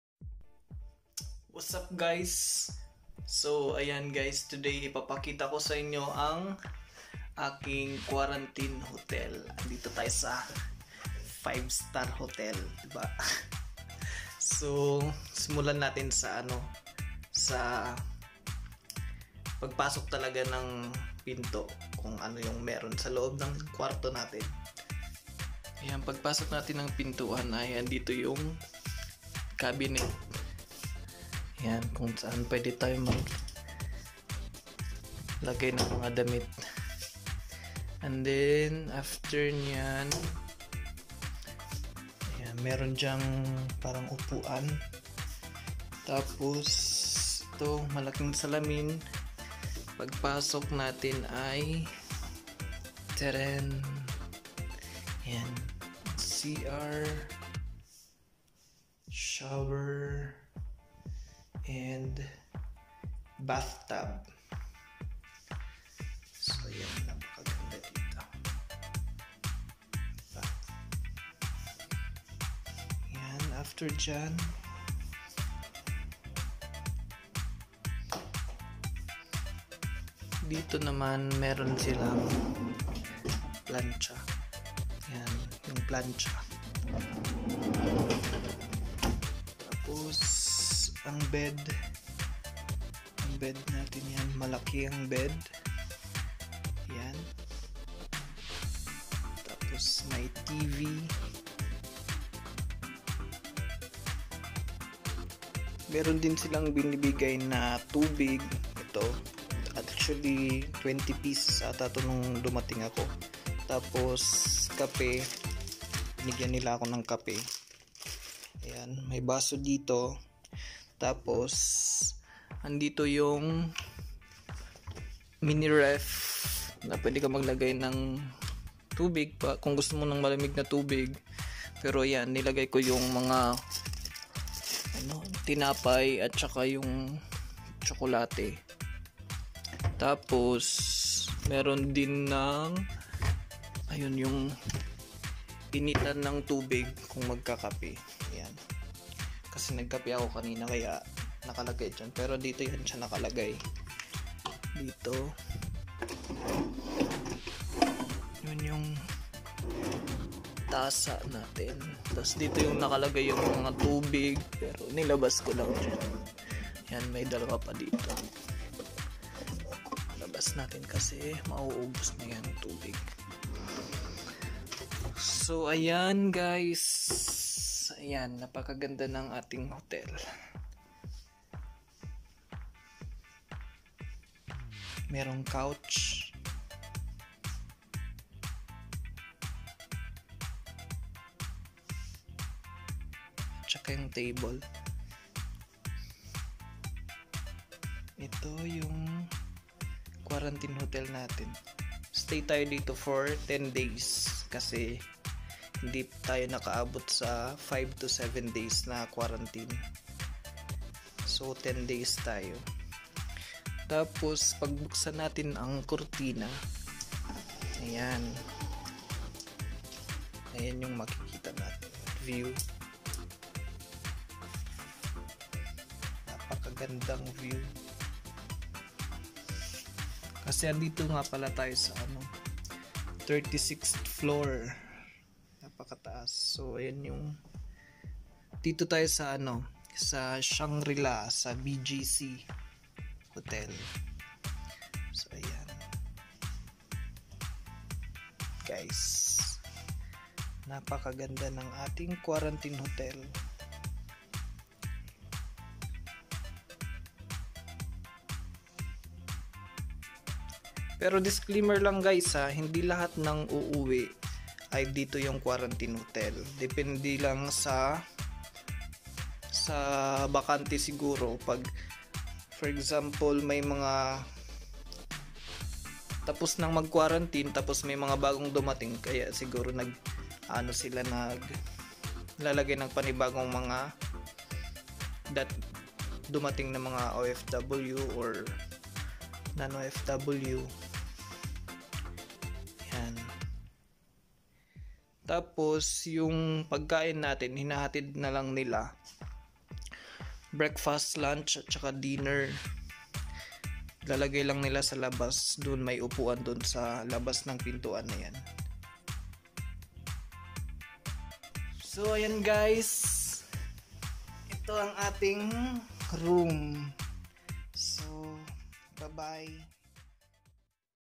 5 STAR HOTEL IN THE PHILIPPINES |SHANGRI-LA
Isa po akong OFW galing sa bansang Saudi Arabia. Noong Oct 9, 2021, umuwi ako upang magbakasyon. Dito po ako sa Shangri-La Hotel sa BGC na quarantine for 10days. Super ganda ng hotel.